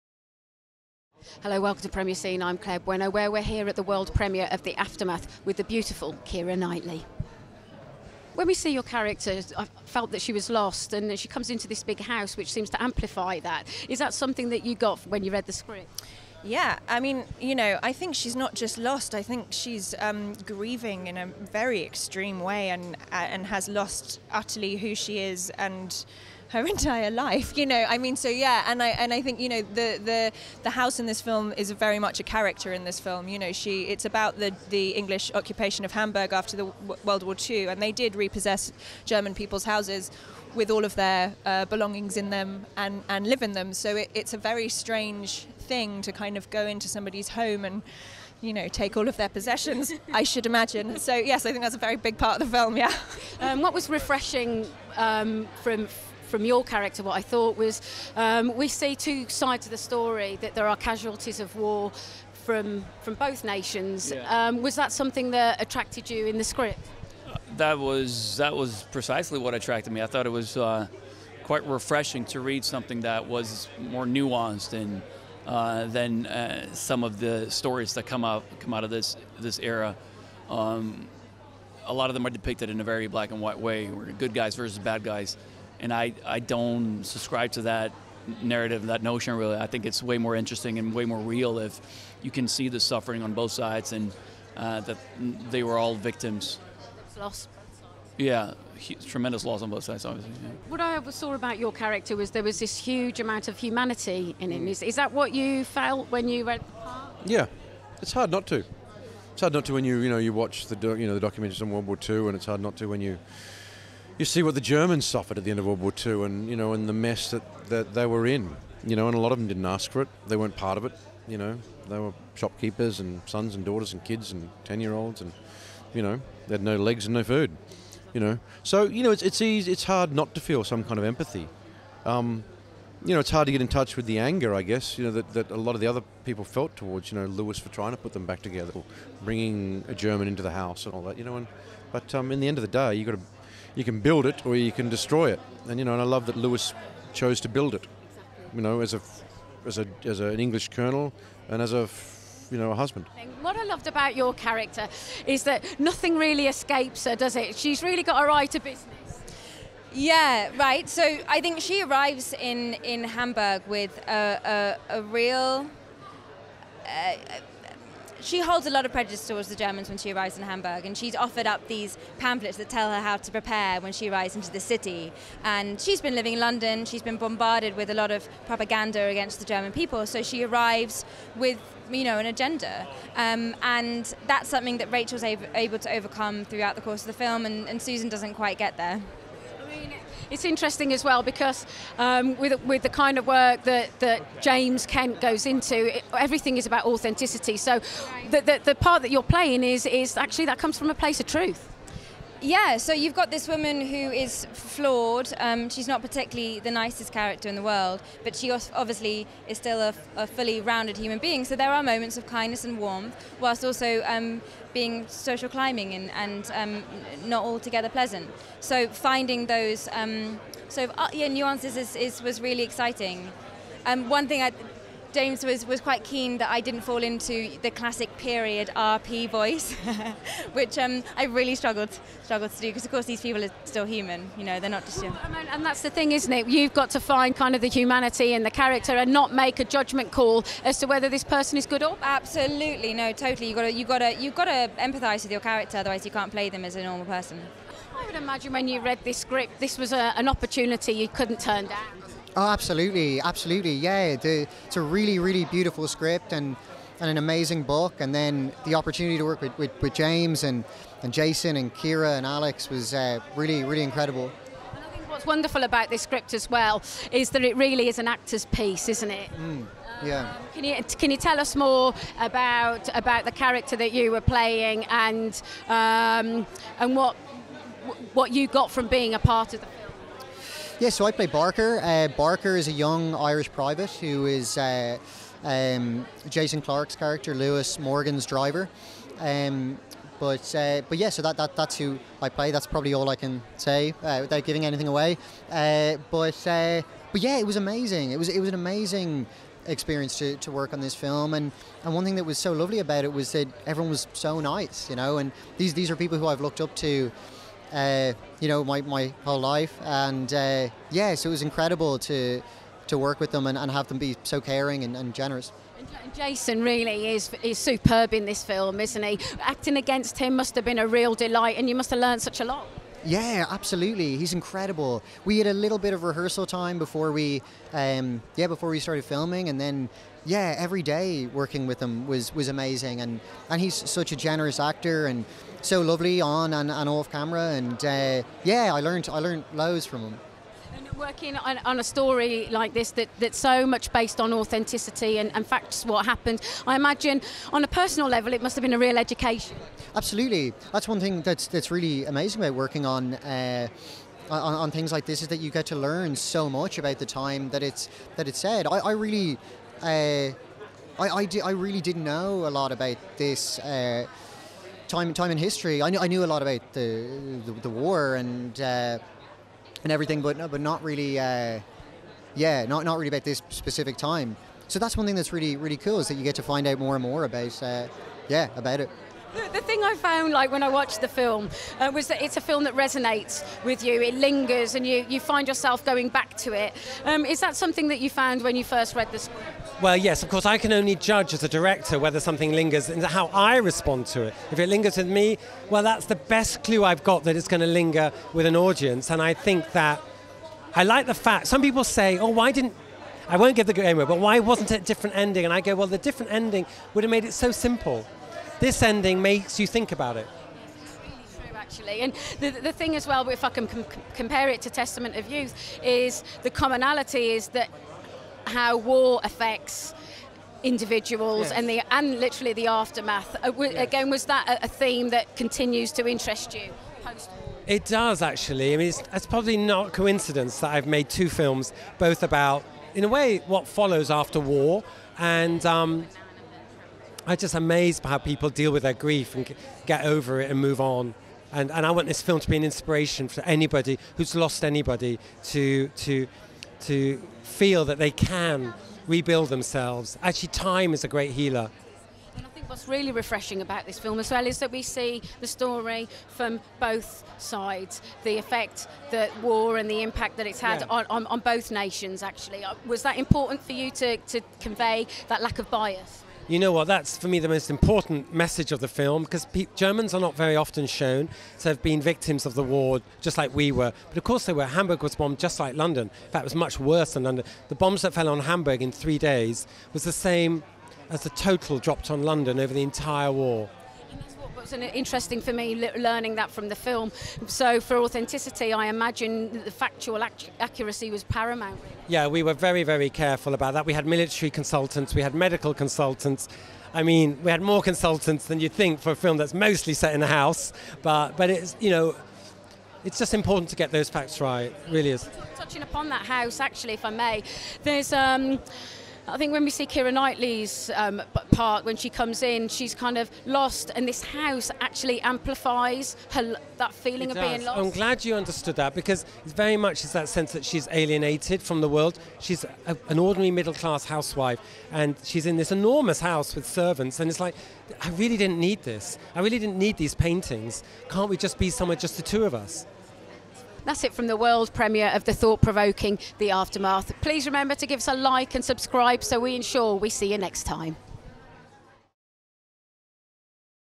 The Aftermath - Keira Knightley, Alexander Skarsgård, Jason Clarke interviews
Set in post-World War II Hamburg, Testament of youth director James Kent delivers a thought provoking film that provides a refreshing point of view on the destruction and devastation caused by war no matter what side you fight for. Adapted from the book by Rhidian Brook.

At the World Premiere of The Aftermath held at The Picturehouse Central Claire Bueno and Anthony Bueno had the pleasure of interviewing actors Keira Knightley, Alexander Skarsgård, Jason Clarke, Kate Phillips, Fionn O'Shea and director James Kent as they attend the red carpet.

Join us as we ask Keira Knightley about her character Rachel Morgan, Alexander Skarsgård about how through his character Stefan Lubert we gain a greater understanding of loss that both nations experience, Jason Clarke about finding the humanity in his character Lewis Morgan, Kate Phillips about the very astute Susan Burnham, Fionn O'Shea about working a historically authentic film and James Kent about directing a story that stays with you.